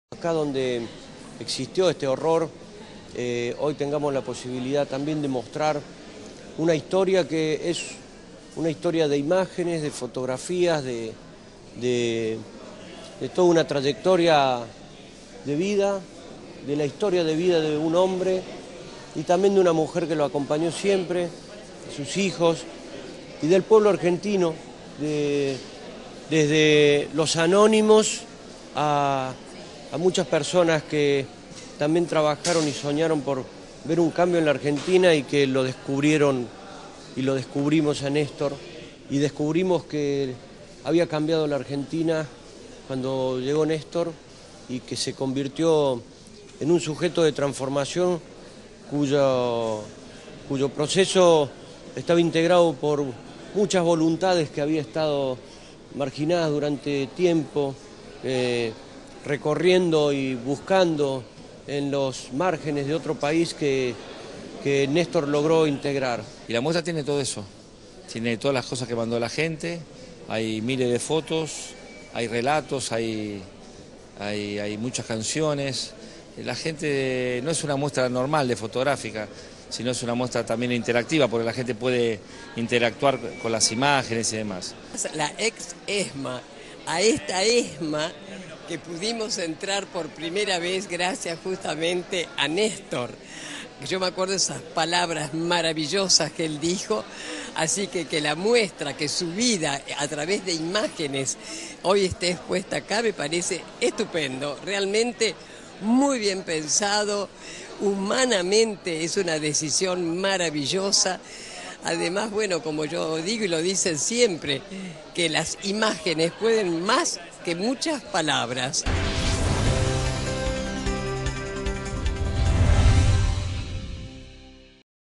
Se inauguró la muestra "Néstor por todos" en la ex Esma
El Secretario de Derechos Humanos de la Nación, Martín Fresneda; la referente de Madres de Plaza de Mayo Línea Fundadora, Taty Almeida; y uno de los productores de la muestra, Jorge "Topo" Devoto, encabezaron el acto de apertura de la exposición fotográfica, audiovisual e interactiva "Néstor por todos", basada en fotografías y otros testimonios enviados por habitantes de todo el país, en el Espacio para la Memoria -ex ESMA.

Fresneda resalto la figura del ex presidente al que se refirió como "un sujeto de transformación integrado por muchas voluntades", y destaco como en la ex Esma un lugar donde existió el horror, ahora se muestra "una historia de toda una trayectoria de vida".

En relación a Néstor Kirchner, Taty Almeida recordó el momento en el que entraron por primera vez a la ex Esma, y que hoy en ese lugar este expuesta su vida "es maravilloso".

La muestra que podrá visitarse hasta el 31 de enero, es resultado de la convocatoria "Vos también podés ser parte de la historia", convocada por los productores Jorge "Topo" Devoto y Fernando "Chino" Navarro con el fin de recopilar material testimonial para la película sobre el ex presidente. "Fue tal el volumen de cosas que decidimos hacer la muestra, y tiene todo eso que mando la gente", aclaro Devoto, uno de los organizadores.